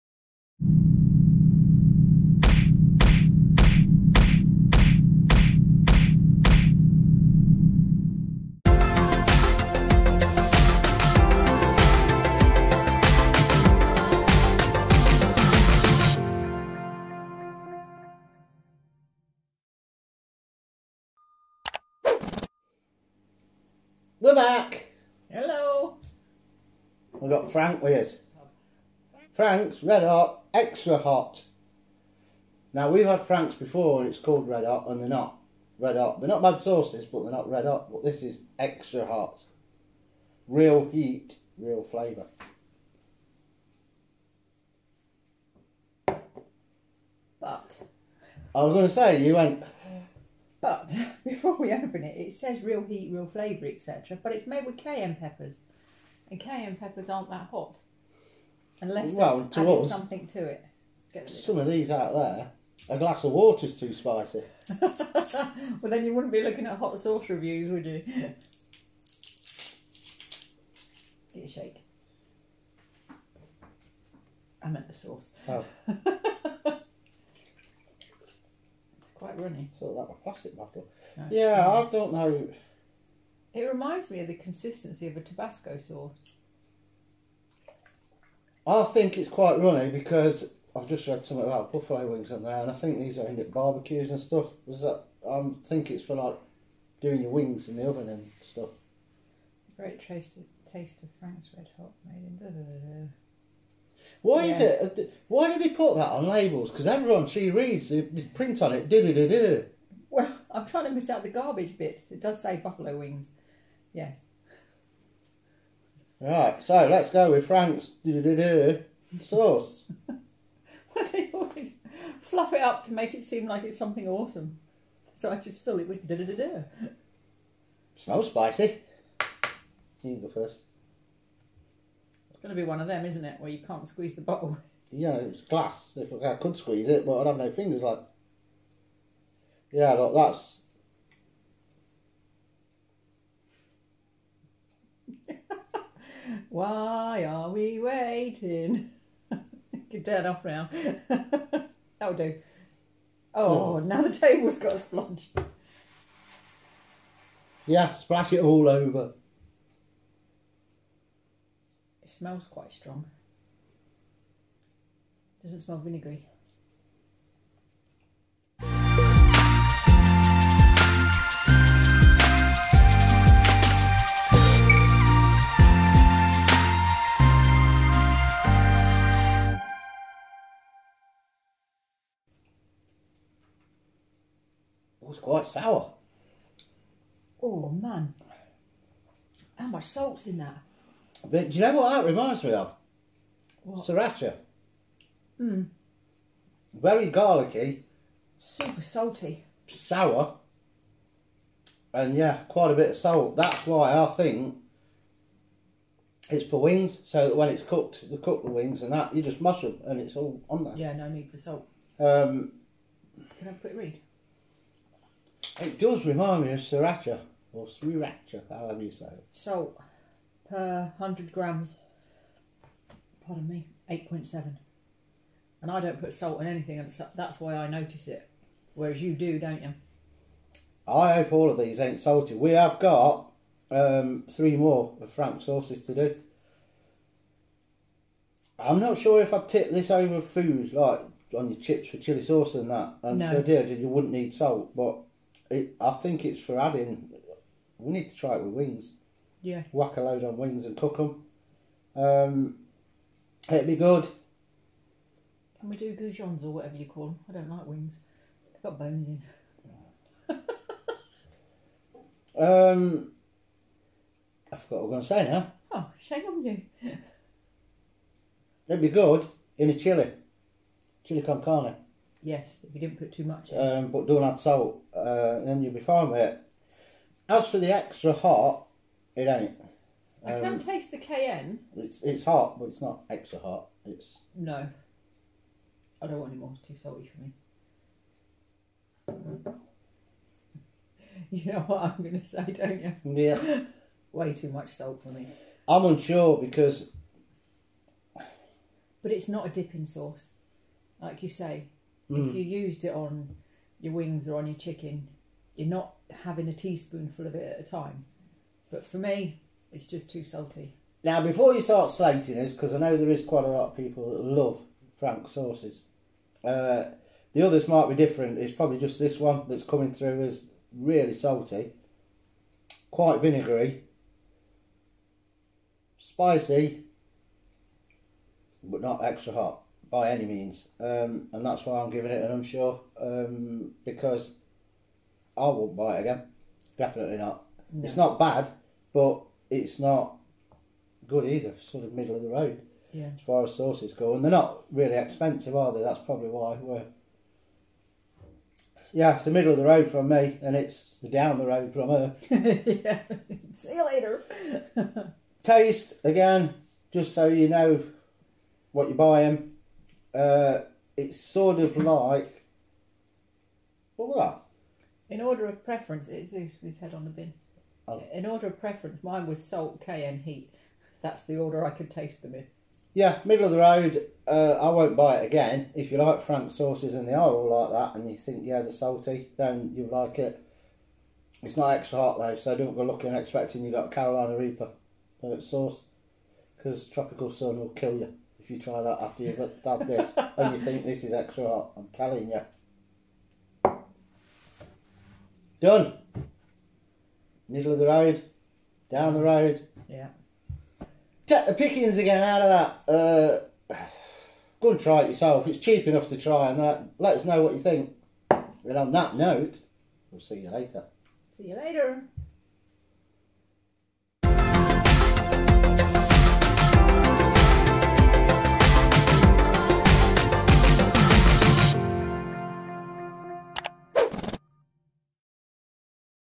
FRANK'S RED HOT | XTRA HOT | CAYENNE PEPPER | SAUCE REVIEW
Well it says Xtra Hot! So let's see shall we?